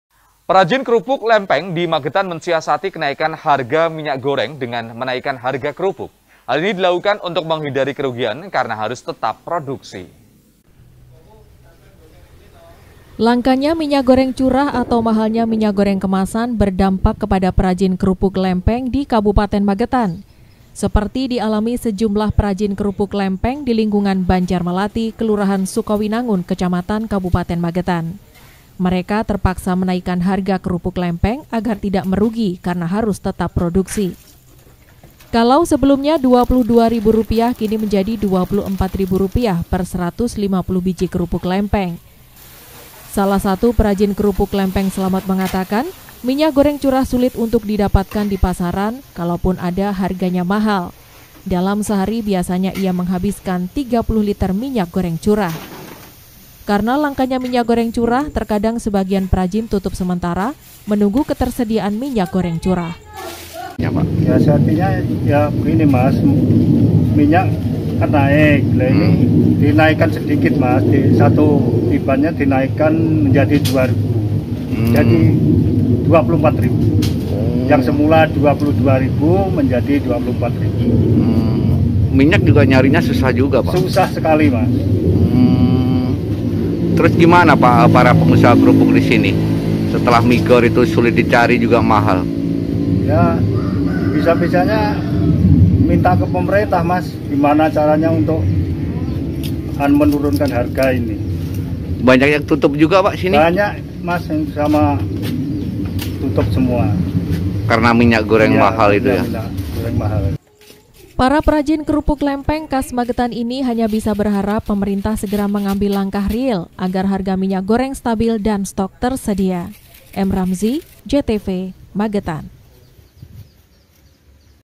MAGETAN - Minyak Goreng Mahal, Perajin Kerupuk Lempeng Naikkan Harga
Perajin kerupuk lempeng di magetan menyiasati kenaikan harga minyak goreng dengan menaikan harga kerupuk. Hal ini dilakukan untuk menghindari kerugian karena harus tetap produksi.